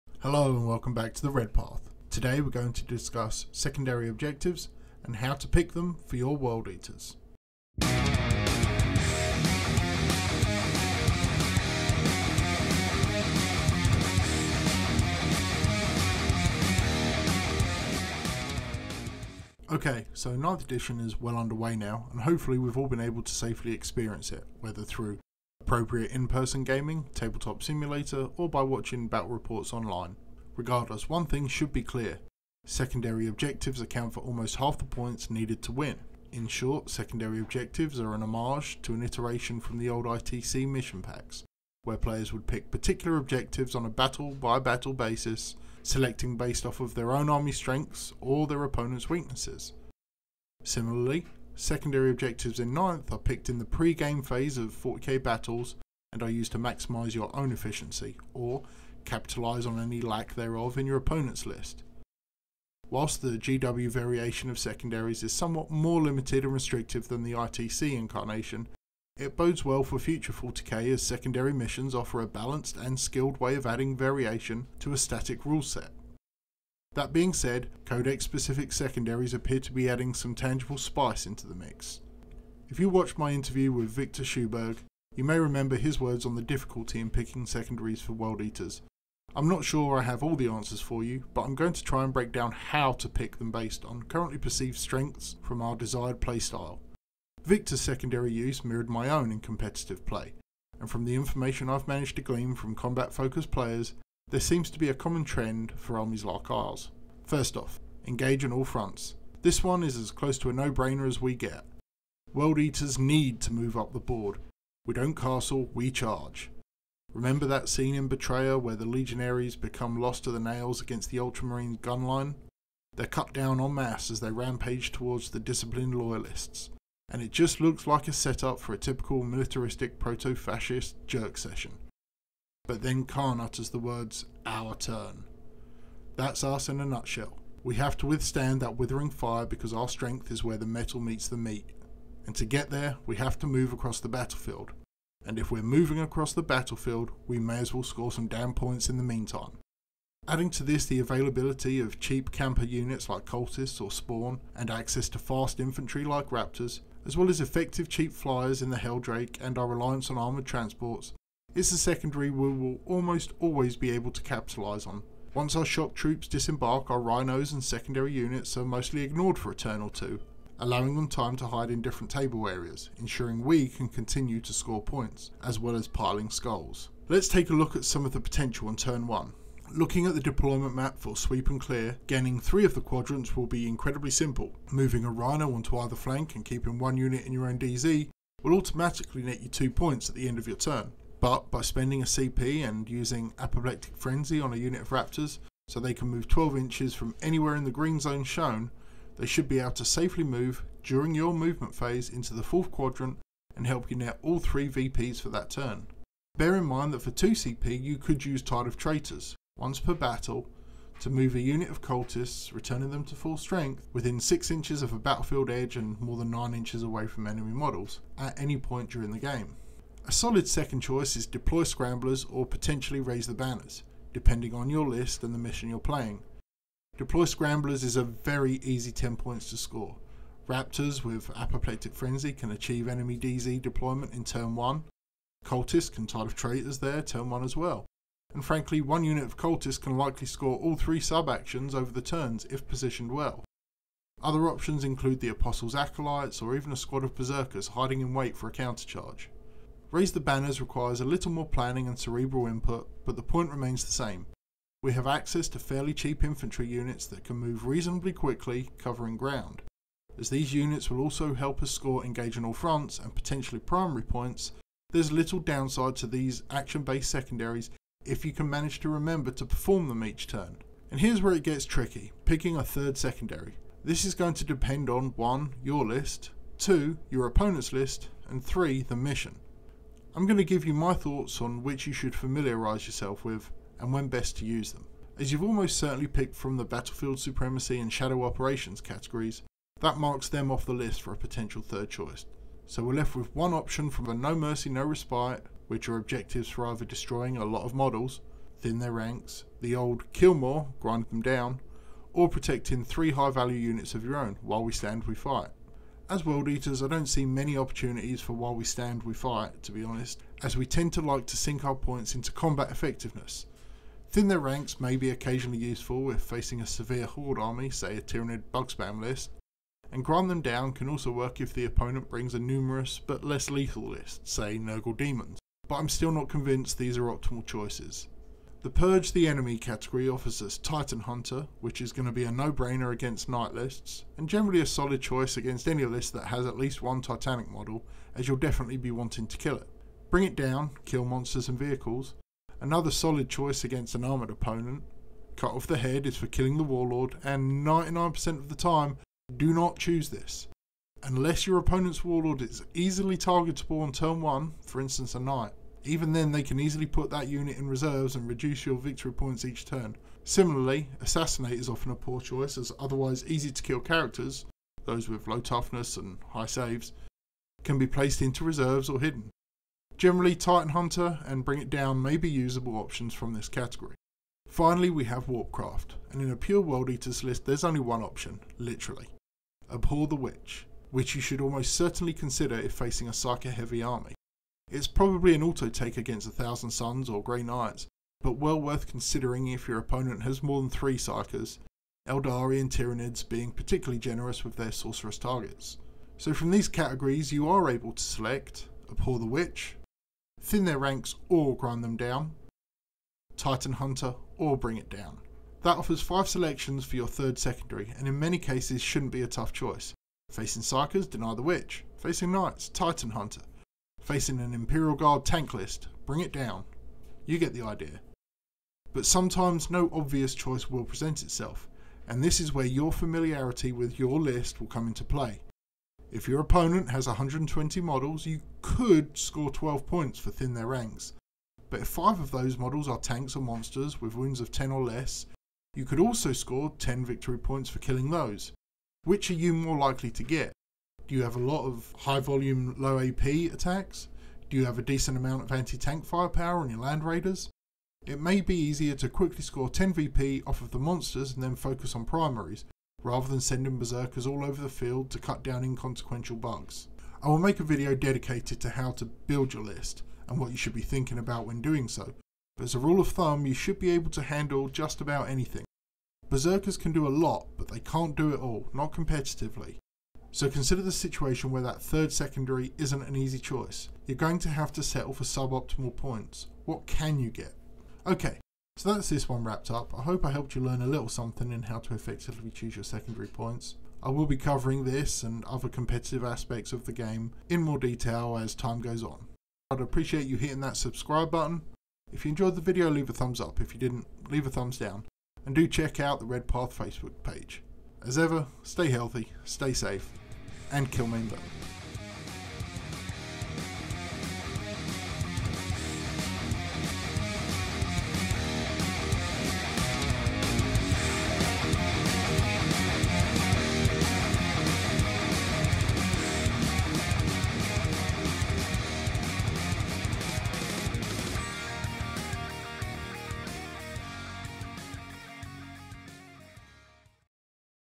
Best Secondaries for World Eaters in 9th Edition 40k
Today we're looking at what the best Secondary Objective choices are for World Eaters in 9th edition, and how to choose them on a case-by-case basis!